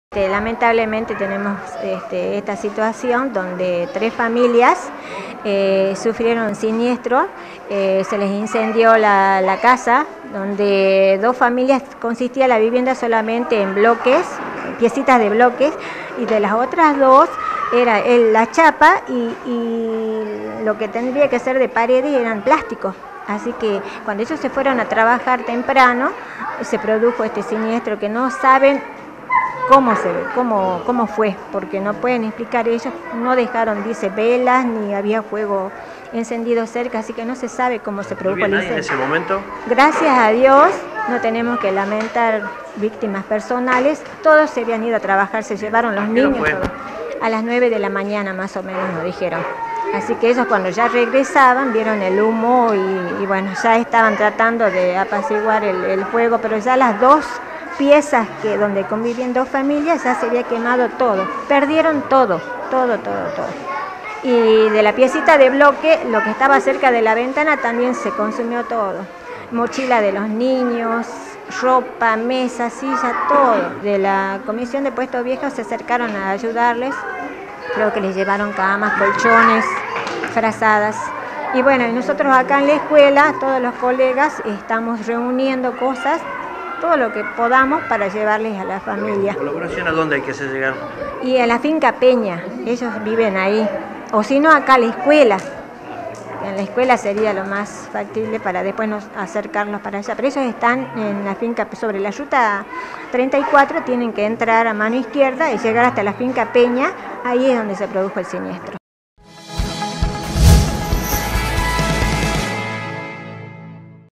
Tres familias de El Carmen sufrieron el incendio de sus viviendas y perdieron todo
Precisan ropa, sillas y mesas. En los hogares residían varios pequeños.